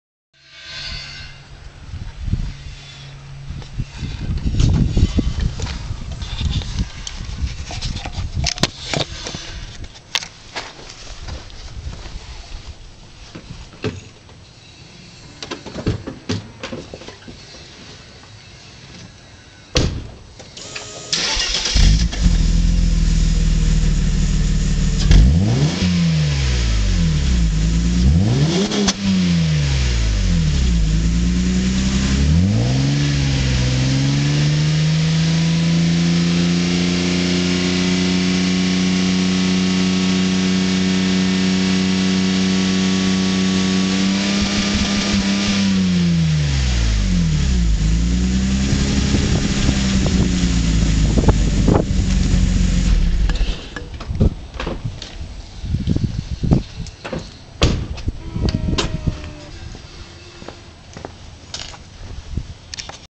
96 Mercedes C280 Sport w202 Lorinser exhaust
As you can see adding a Lorinser exhaust
doesn't mean you're going to have something too loud.
I wanted something other than stock but not sound loud like a coffee can import muffler (tip: your neighbors don't like loud mufflers)